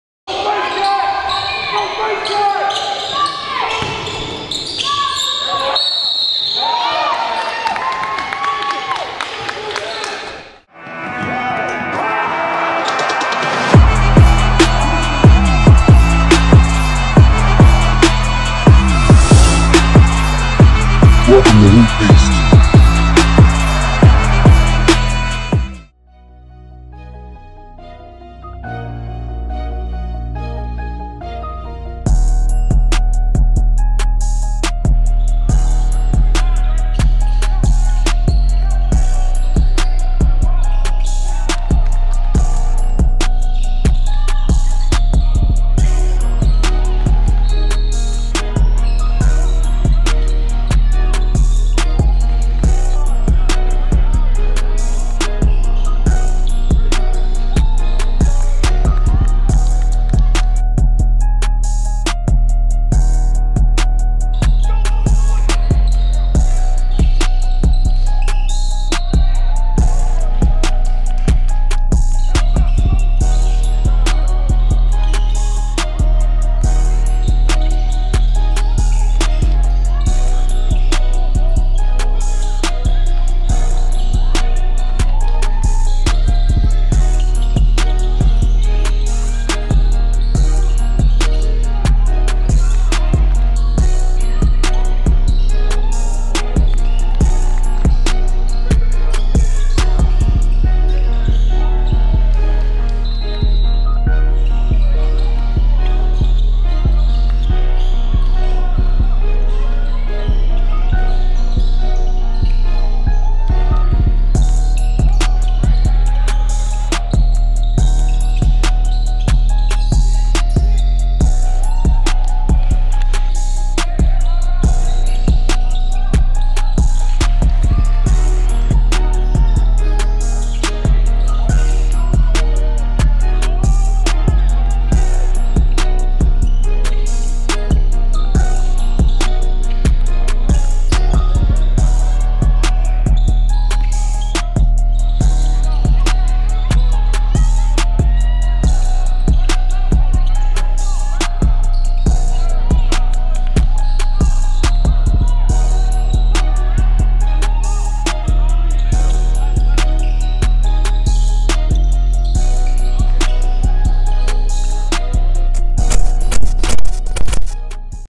Just hoopin’
28pts 12rebs 6 assists vs. TSF EYBL @OTR Elite 32. Josiah Lawson c/o 2024